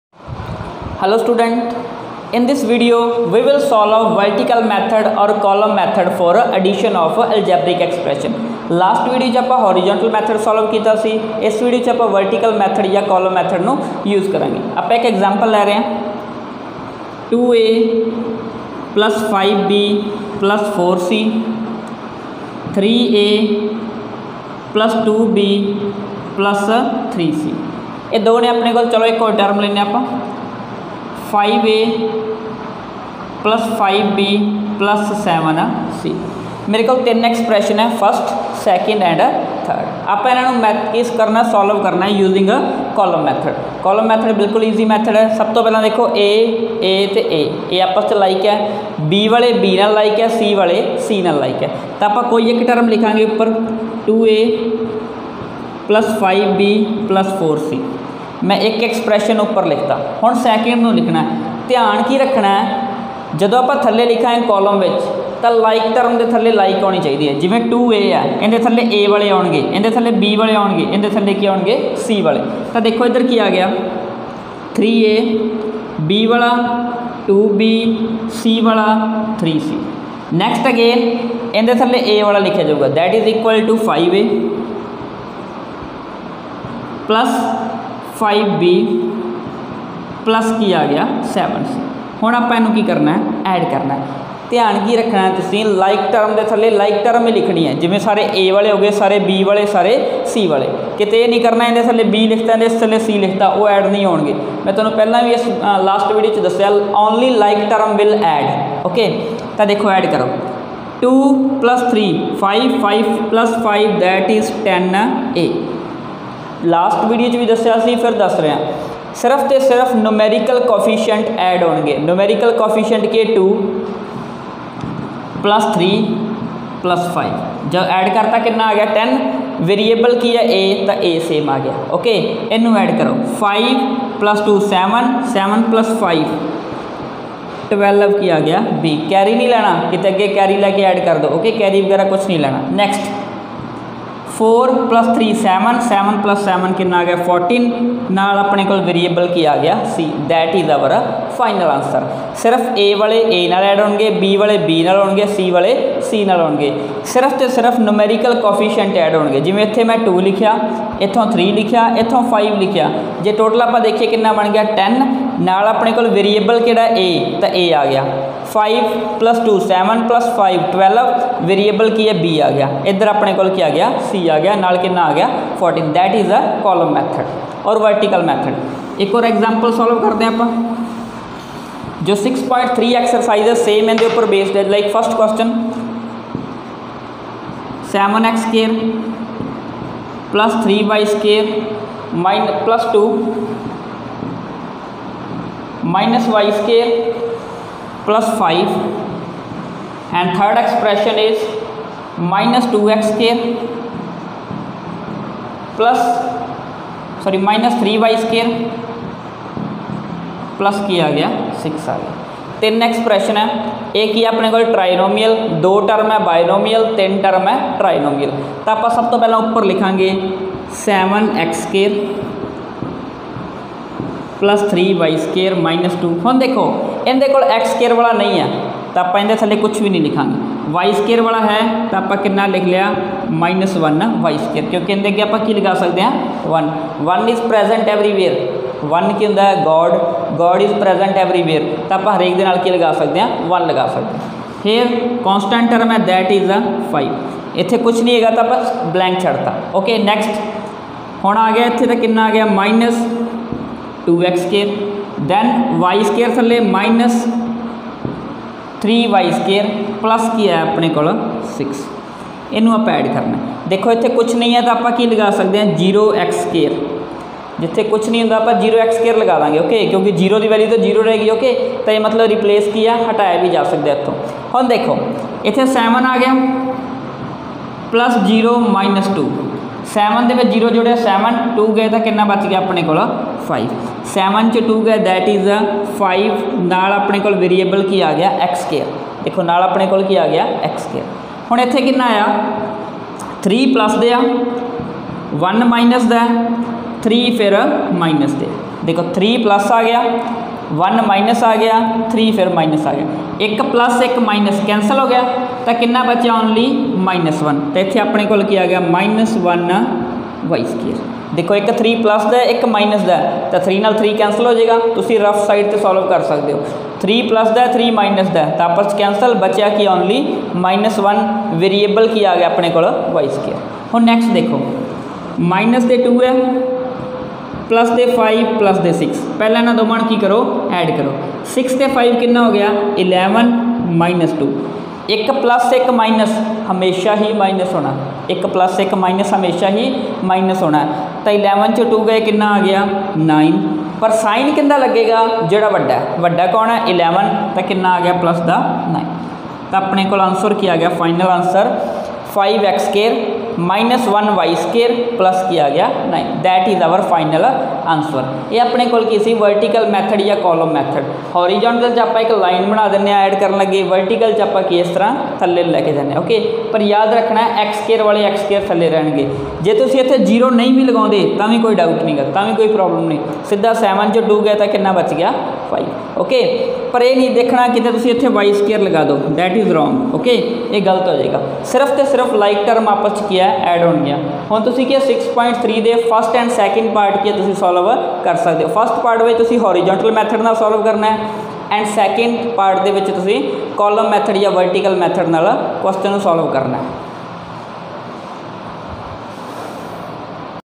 Vertical method to add Algebraic expression|class 7|chapter 6|exercise 6.3|Maths mate book|part 2
We have discussed vertical method to afd Algebraic expression